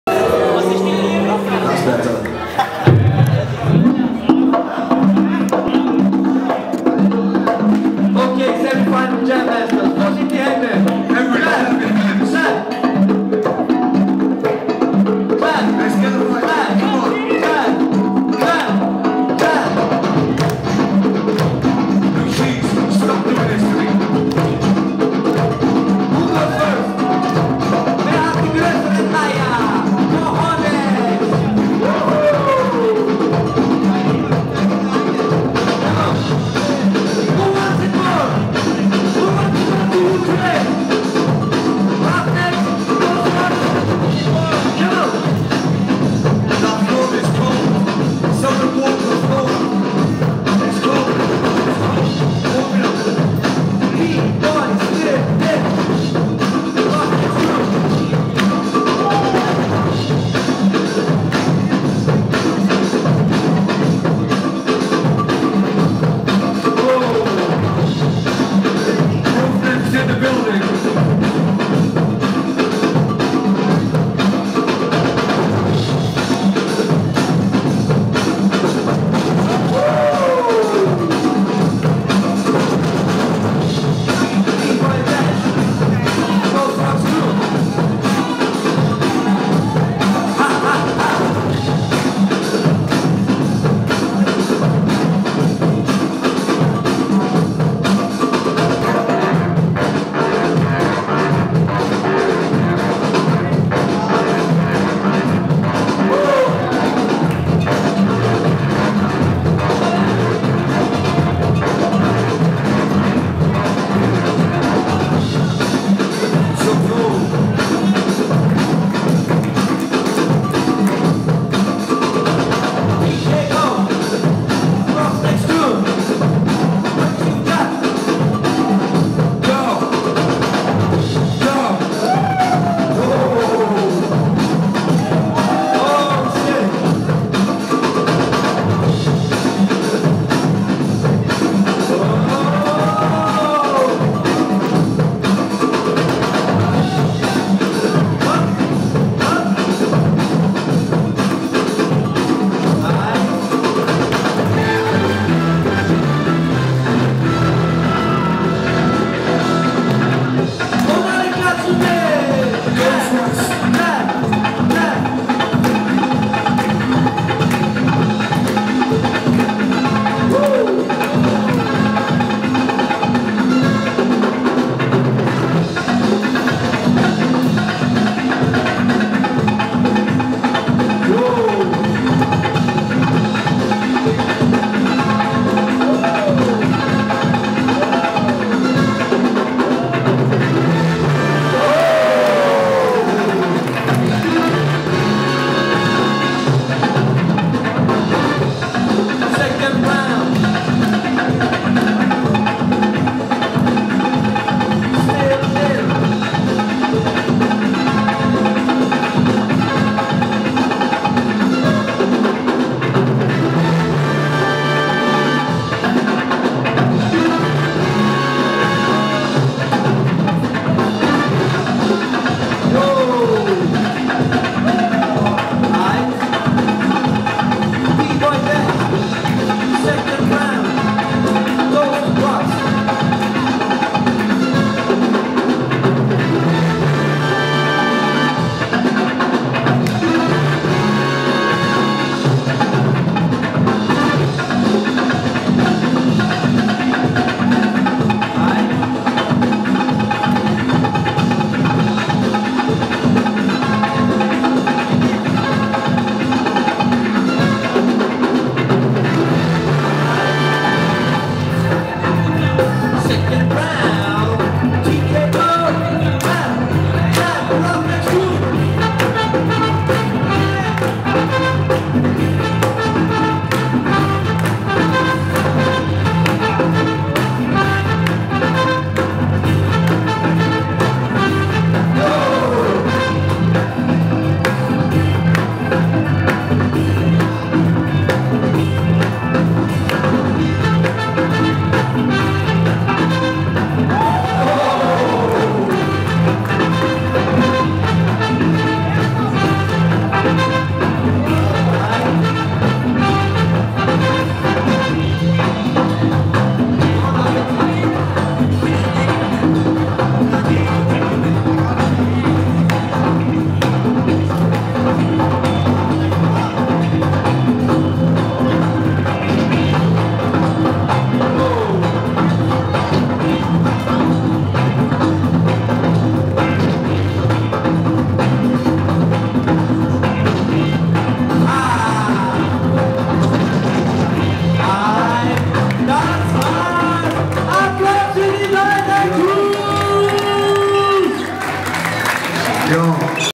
ORIGINAL FLAVOUR VOL 1: GHOST ROCKZ vs RUFF'N'X
ORIGINAL FLAVOUR VOL 1 - 2012 - FLOORMASTERZ QUALIFICATION:

GHOST ROCKZ (SON LE & DASH) vs RUFF'N'X (T-KO & STILL'ILL)